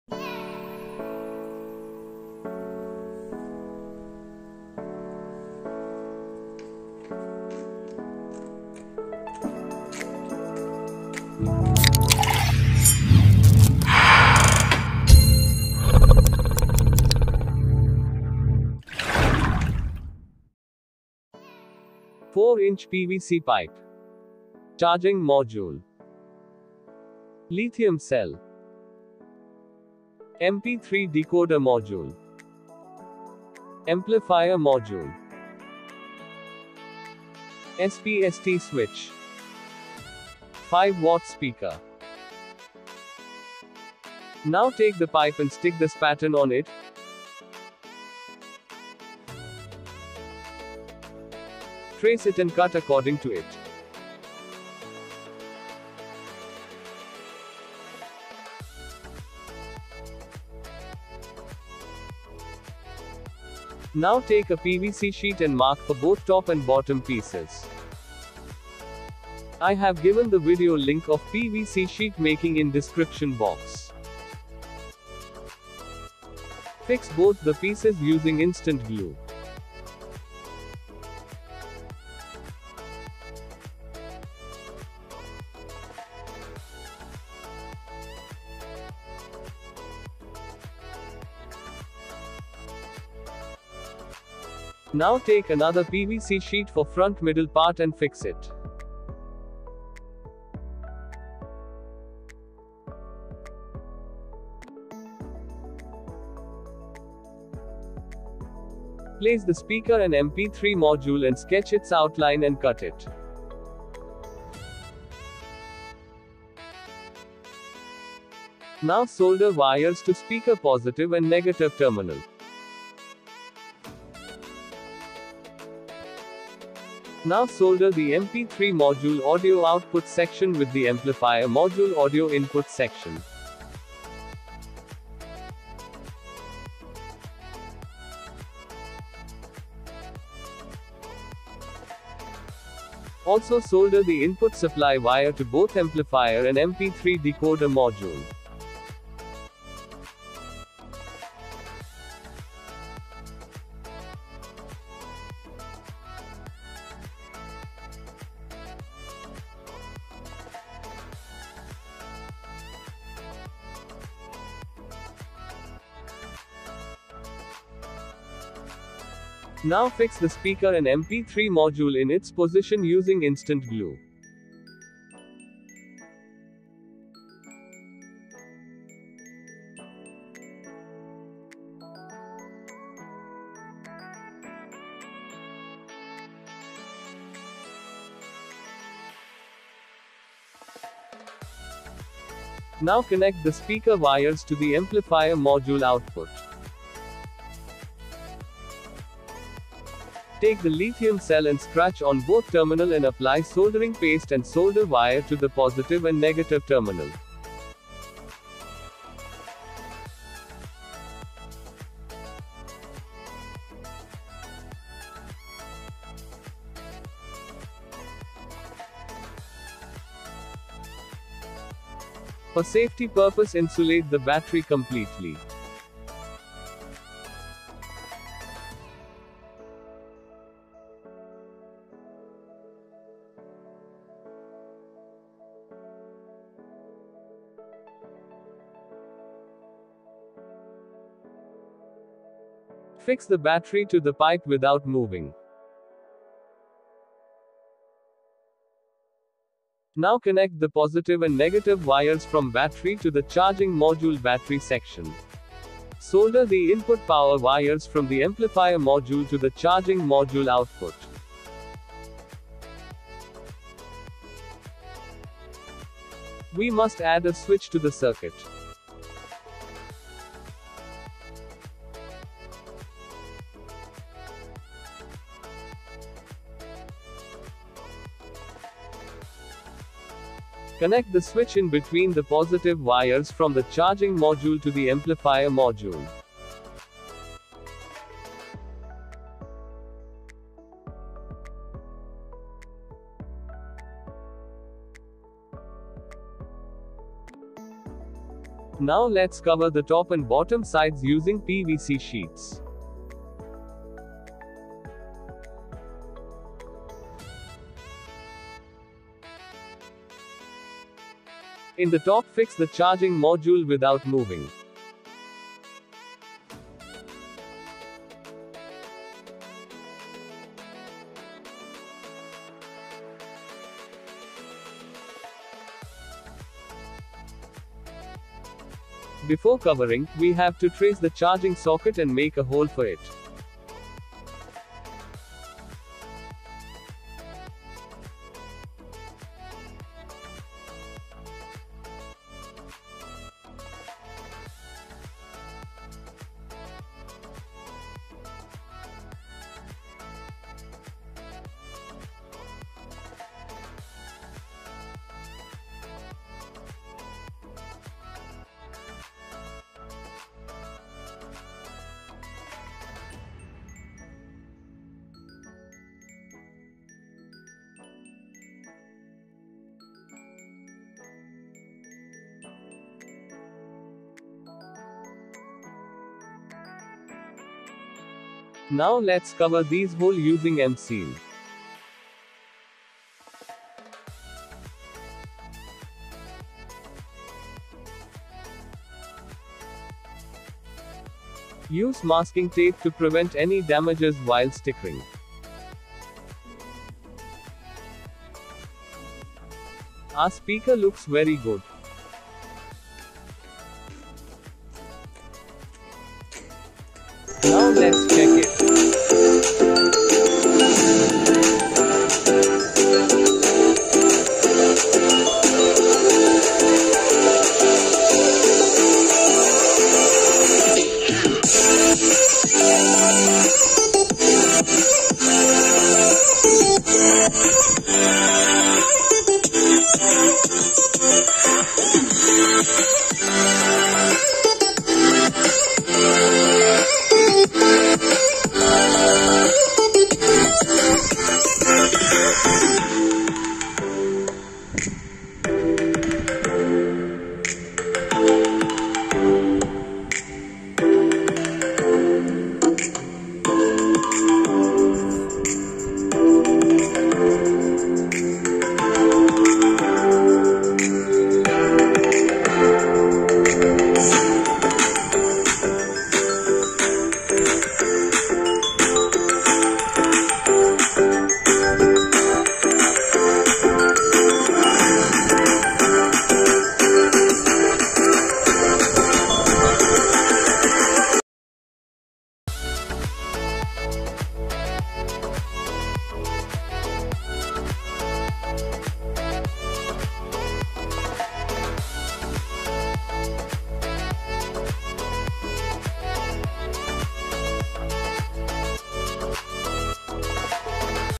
DIY Bluetooth speaker | usb | fm | sd card | aux
Hi Friends
                In this video we are going to make a multifunctional portable speaker which has bluetooth, fm, sd card, aux support with remote functionality. ( Bluetooth speaker diy )

0:00 SPEAKER ROLLOUT
0:20 COMPONENTS REQUIRED
0:42 MAKING
6:36 AFTER STICKERING
6:50 TESTING

               we use 4-inch pvc pipe to make the body of the speaker and also we use pvc sheets. The speaker consists of a mp3 bluetooth decoder module (PAGARIA), 5W speaker, 2500mah lithium ion battery with tp4056 module to charge the battery. This speaker uses pam8403 amplifier module.

               The build quality of the speaker is solid and the design looks attractive, easy to carry and less weight.

               The speaker has multiple functions like bluetooth, fm, sd card, aux modes and it comes with a remote by using it we can control the full functionalities of the speaker.

              You can connect this speaker with your smart devices via bluetooth and also you can connect other devices by using aux cable.

              The battery used here is 18650 lithium ion battery of 2500mah, which has good life and gives good backup of more than 8+ hours of backup in 60% volume

             The speaker used here is gives good sound with high bass with no noise or humming issues.

             TP4056 charging module is used to charge the battery which has may protections like discharge protection, and charging current regulations, so our speaker becomes completely protected.

              We use a single spst switch to control the power on and off of the speaker.